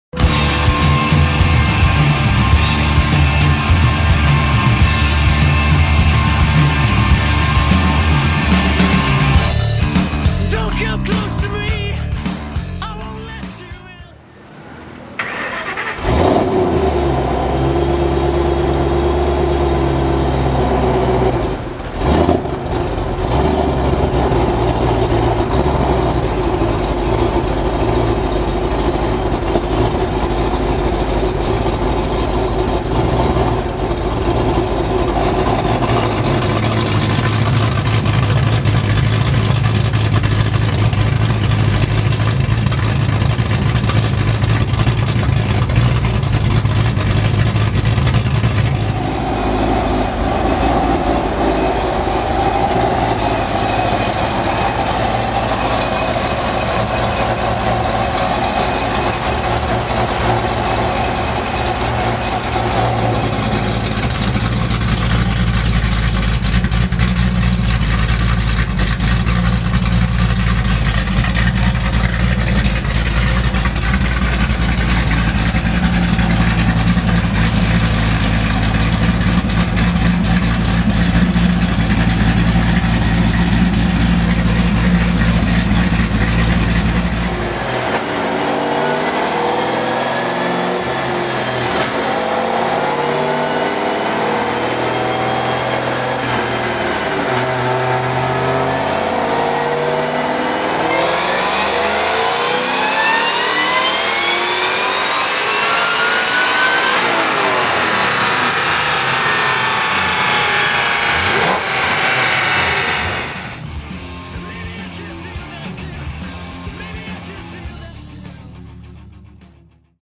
2011 Kenne Bell Roush Mustang GT with L&M Custom Cams - built by Power by the Hour
This is Andy's 2011 Roush Mustang GT featuring a Kenne Bell 2.8L blower, L&M Longblock with Custom Cams and Lund Racing Tune.